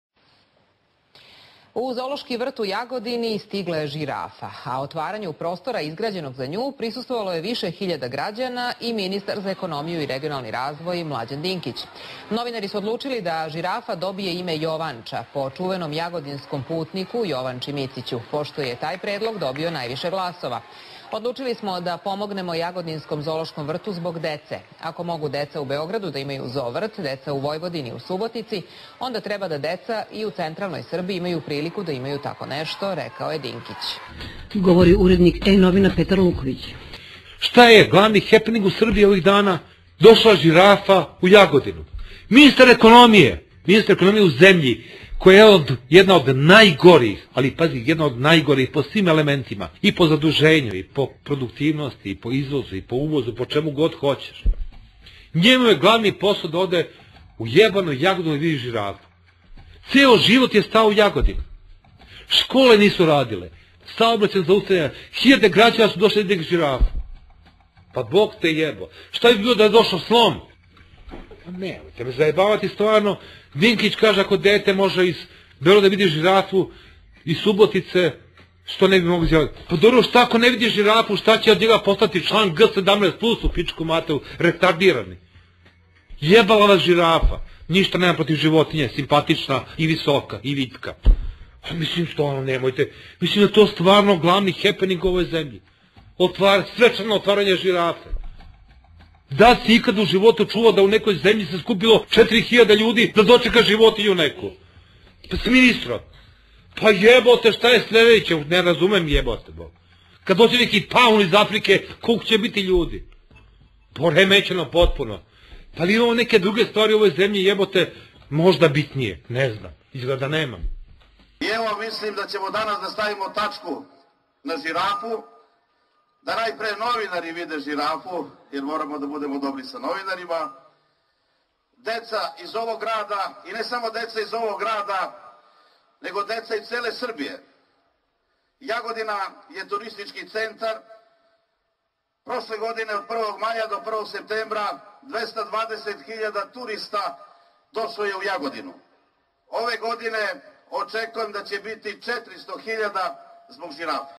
O zirafi Palmi Mladji ....wmv
Petar Lukovic o Mladji, Palmi i "najvažnijem" dogadjaju za buducnost dece Jagodine i uže Srbije koja su do sada bila diskriminisana, ali zahvaljujuci prvoj dvojici sada imaju jednaku sansu da vide zirafu kao i deca iz Vojvodine i Beograda